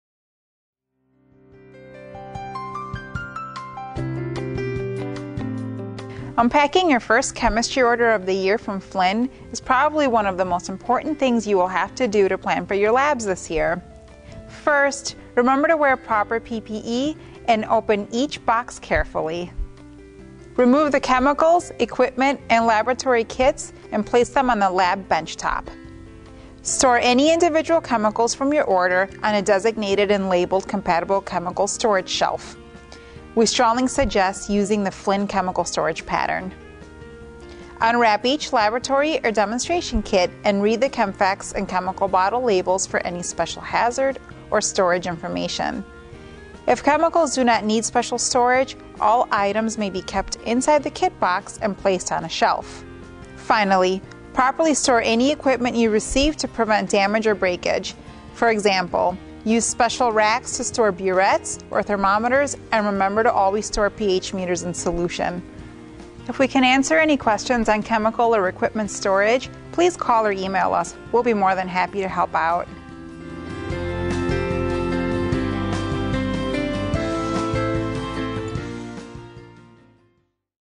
Unpacking Your Chemistry Order from Flinn
Whether you order chemistry supplies just once a year, or throughout the year as needed, these unpacking tips can help keep your supplies safely organized.